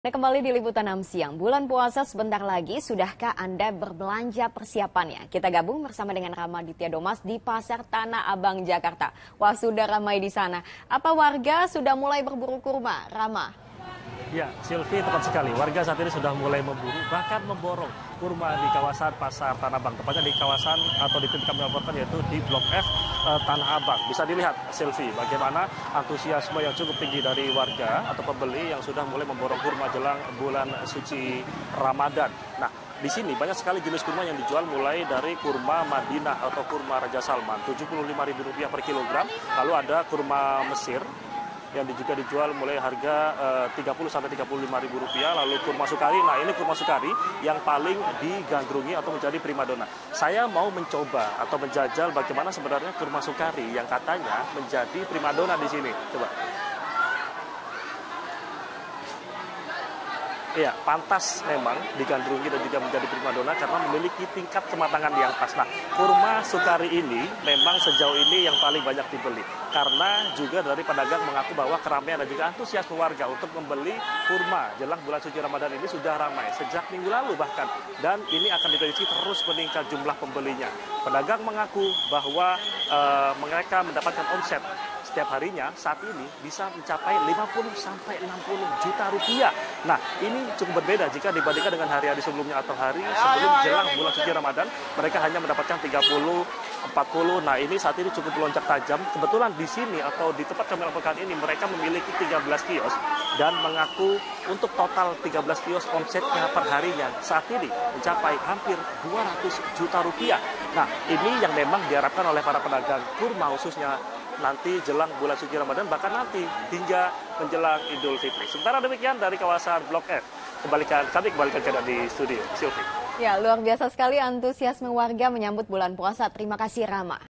Jelang Ramadan, Warga Antusias Berburu Kurma Jenis Mesir dan Sukari | Liputan 6
Bulan puasa sebentar lagi. Sudahkah anda berbelanja persiapannya? kita bergabung bersama Ramaditya Domas di Pasar Tanah Abang, Jakarta.